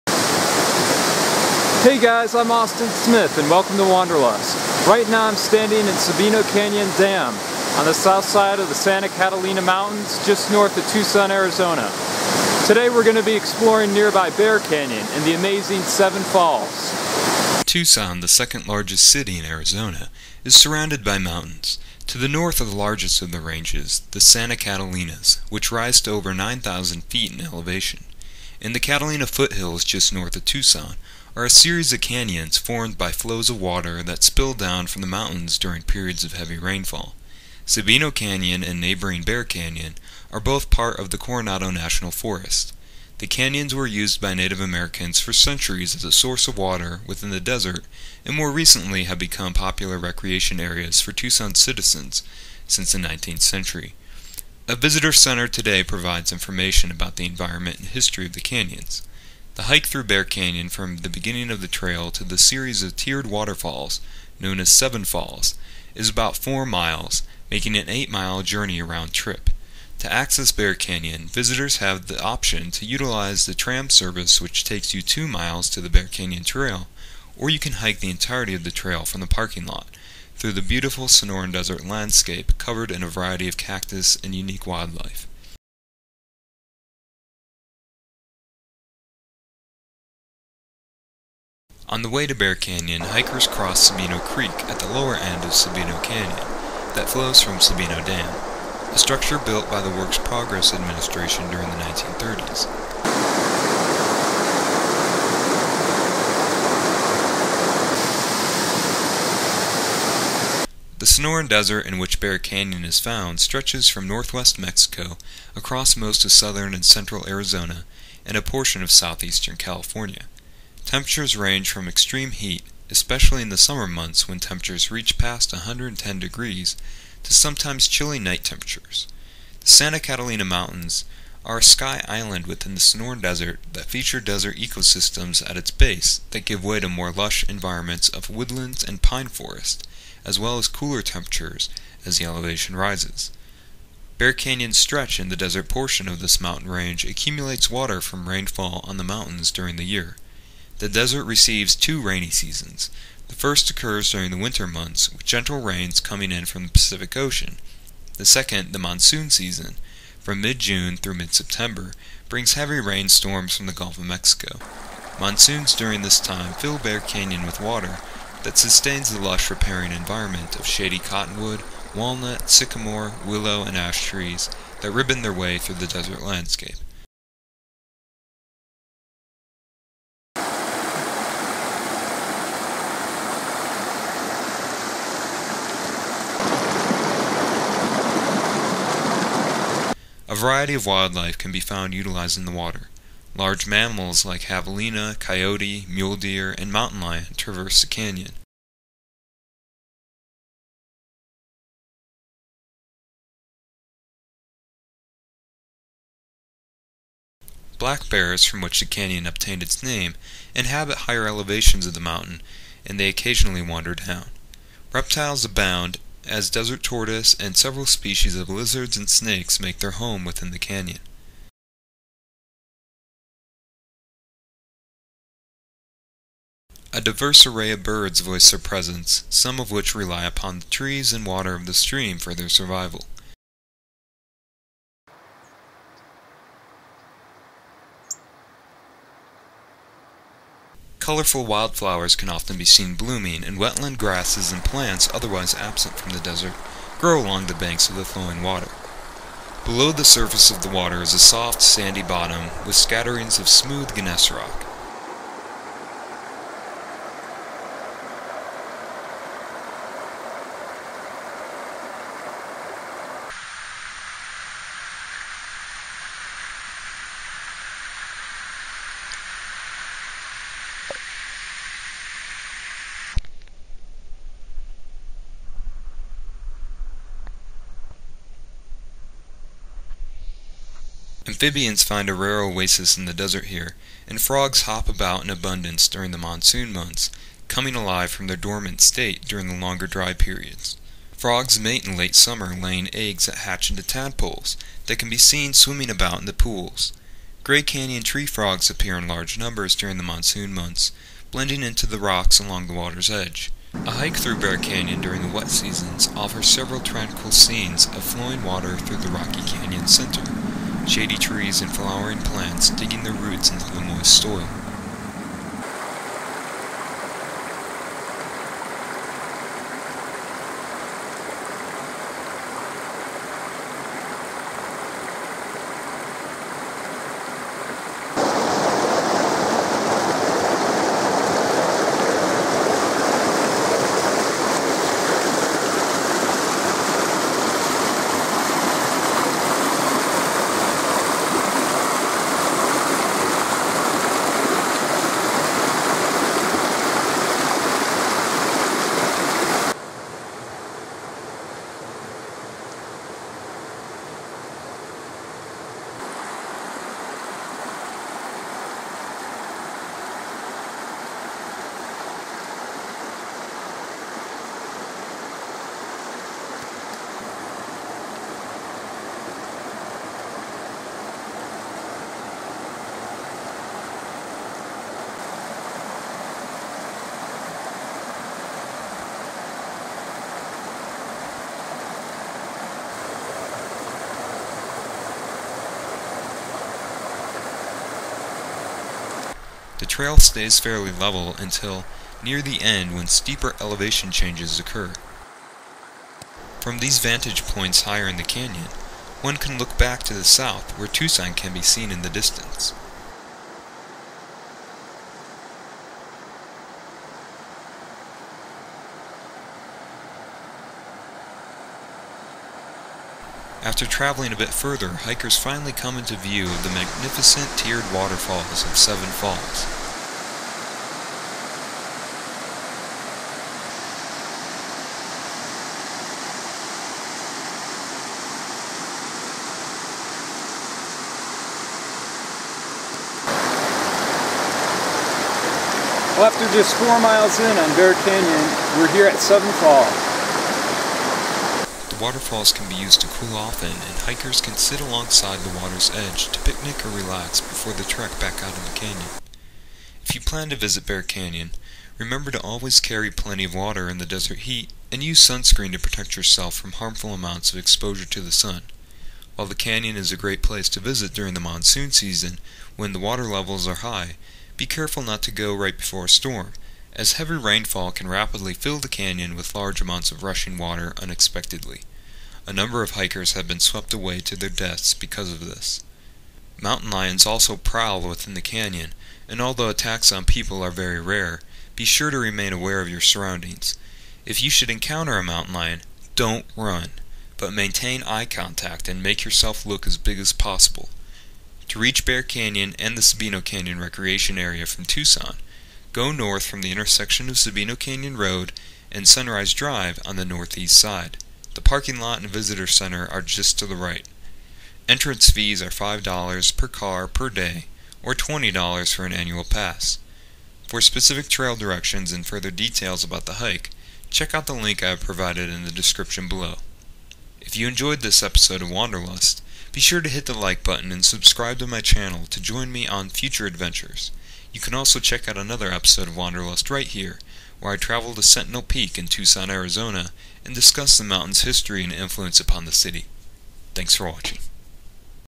Bear Canyon & Seven Falls near Tucson, Arizona
Travel with me to Bear Canyon and Seven Falls in the Santa Catalina Mountains near Tucson, Arizona. The canyon here fills with water during the rainy seasons and becomes lush with greenery and wildlife during the monsoon months. Javelina, coyote, bobcat, mule deer, mountain lions, desert tortoise, canyon tree frogs, and a variety of lizards, snakes, and birds make their home here. Colorful wildflowers, wetland grasses, and shady trees, including cottonwood, walnut, sycamore, willow, and ash grow in a ribbon along the flowing water's banks. The sandy bottom of the canyon beneath the surface of the water is scattered with smooth gray gneiss rock. The series of seven waterfalls at the end of the 4 mile hike (8 miles roundtrip) offer a great place to relax, picnic, and cool off from the heat.

My trip to Bear Canyon and Seven Falls in the video took place in the morning of Friday, September 13, 2013.

Photos Credits (all public domain or released for use):
Coyote. Susan Lynn Peterson, 2007.
Collared peccary. Alan Vernon, 2011.
Collared lizard. MostlyDeserts, 2012.
Curve-billed thrasher. Alan Vernon, 2011.
Western bluebird. berichard, 2010.
Cactus wren on a saguaro cactus. Alan Vernon, 2011.
Canyon treefrog. Magnus Manske, 2009.
Tadpoles. bohringer friedrich, 2008.